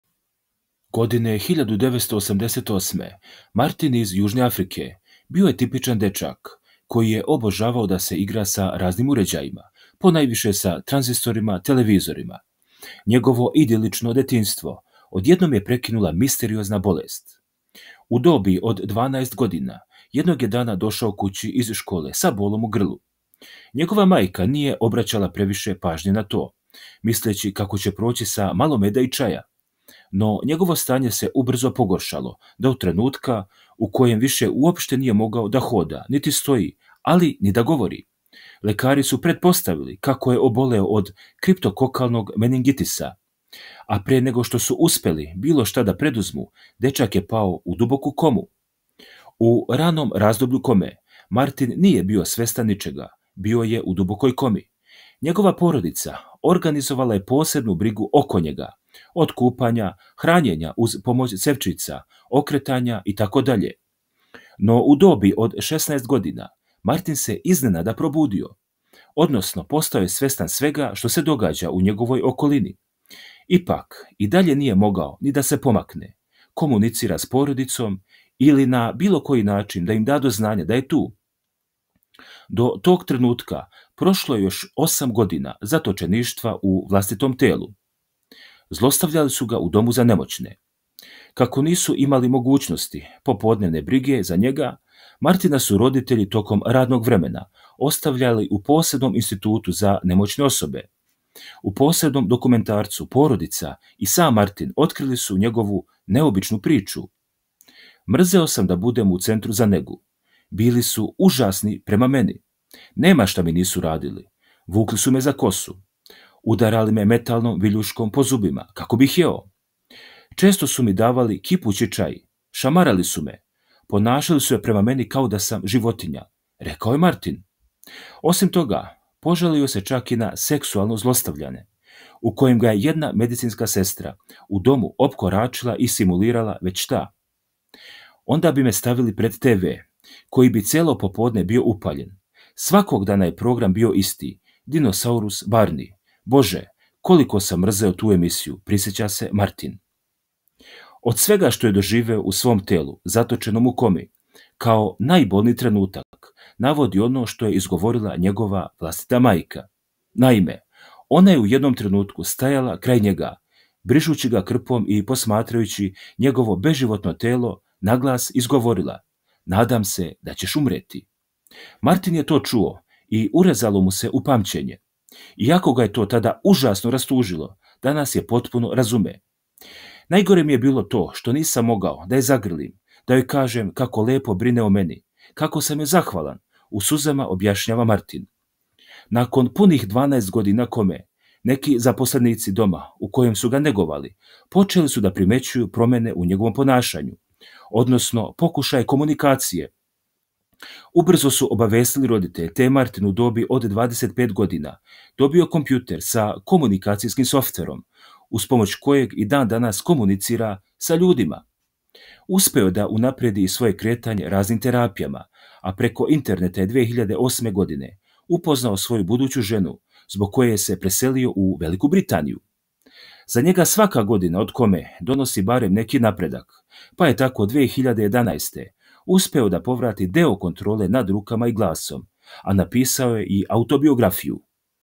Probudio se Posle 12 Godina iz Kome i Otkrio Užasne Stvari Koje su mu Radili
Prijavite se na Rijaliti Kanal i hvala na poseti!
Podelite ovaj video na nekoj od društvenih mreža obavezno!